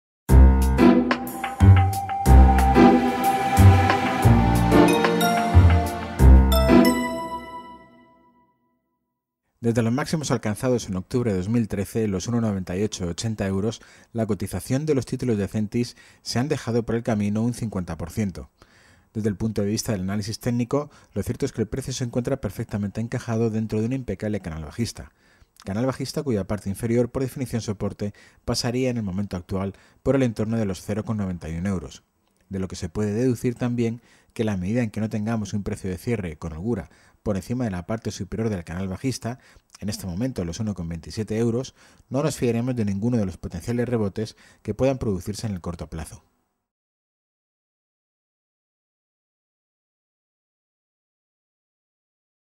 Ezentis: sigue encajado dentro de un impecable canal bajista
Desde los máximos de 2013 el título acumula una caída del 50%.

Además, se puede observar cómo en los últimos meses el precio se encuentra encajado, con máxima precisión, dentro de un impecable canal bajista y, por lo tanto, a día de hoy es un título que no interesa desde el punto de vista chartista.